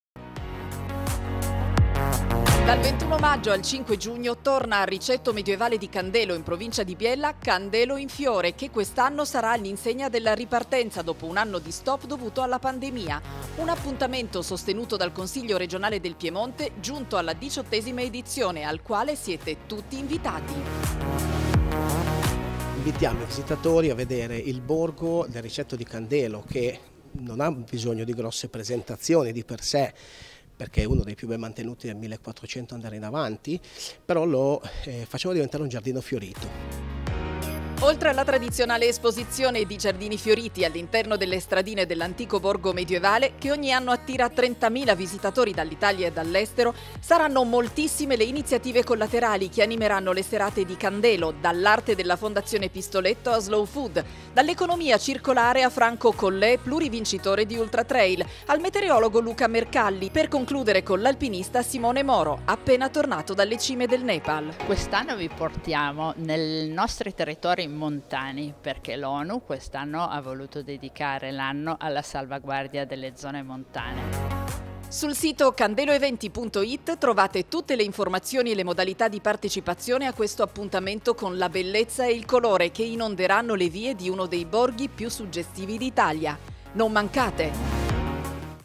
Candelo in Fiore
Dal 21 maggio al 5 giugno il Ricetto medioevale di Candelo (vicino a Biella) tornerà a fiorire, anche grazie alla partecipazione del Consiglio regionale del Piemonte.